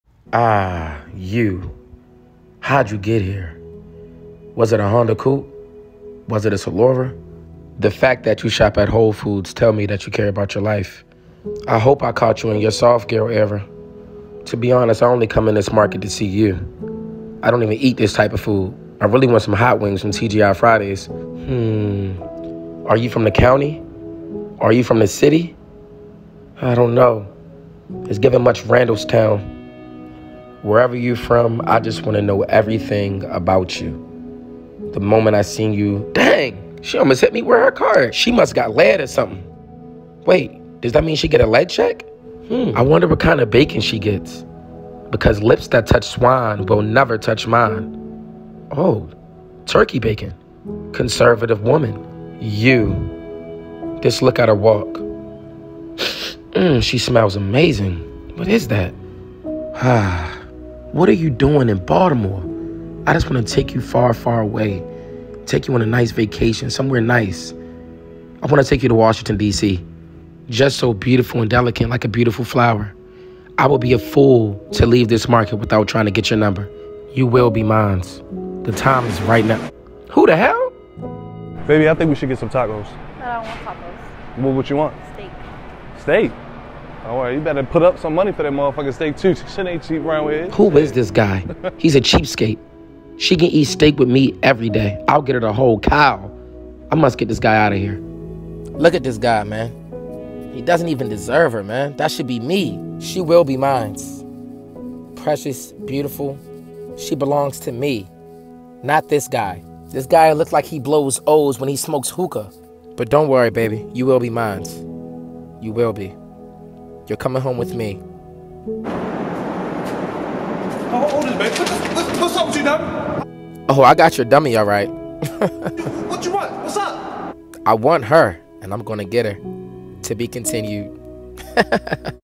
Yeww (Part 1)
A funny spin off to the original “YOU” with a Baltimore twist